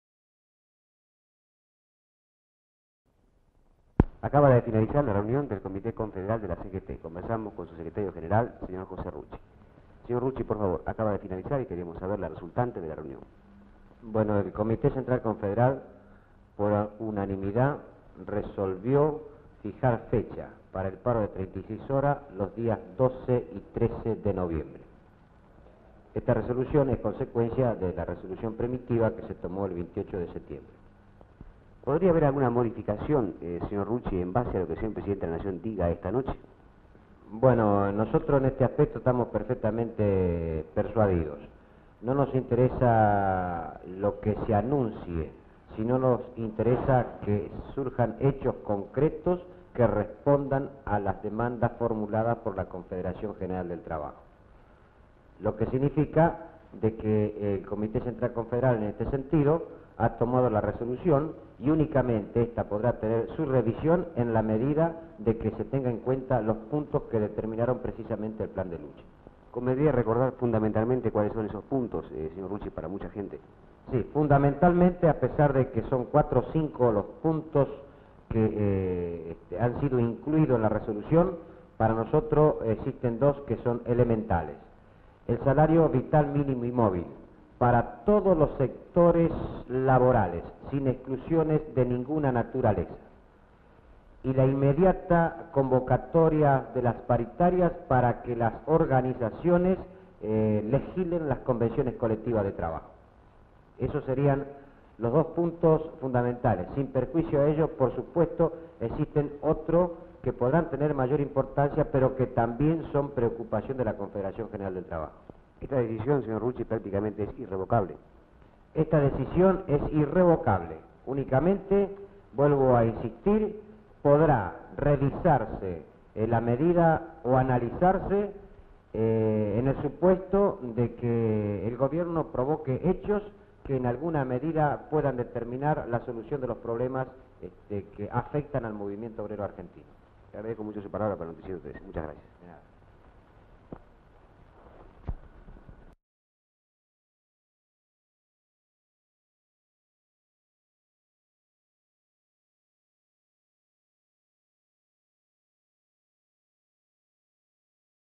DiFilm - Jose Rucci paro nacional de 36 horas resuelto por la CGT 1970
Bs. As.: Reportaje al secretario general de la Confederación General del Trabajo (CGT), José Ignacio Rucci, en el cual se le pregunta: -¿Acaba de finalizar y queremos saber lo resultante de la reunión que mantuvo con el comité confederal de la CGT? (Responde: El comité central confederal por unanimidad resolvió fijar fecha para el paro de 36 horas los días 12 y 13 de noviembre. Esta resolución es concurrencia de la resolución primitiva que se tomo el 28 de septiembre) -¿Podría haber alguna modificación en base a lo que el presidente de la Nación diga esta noche? -¿Convendría recordar fundamentalmente cuales son esos puntos señor Rucci para mucha gente? (Responde: El salario vital mínimo y móvil para todos los sectores laborables y la inmediata convocatoria de las paritarias para que las organizaciones legislen las convenciones colectivas de trabajo) -¿Esta decisión prácticamente es irrevocable? (Tico Rodríguez Paz)
Fecha: 27/10/1970
Duración: 2 minutos 25 segundos
Código del film: B-01247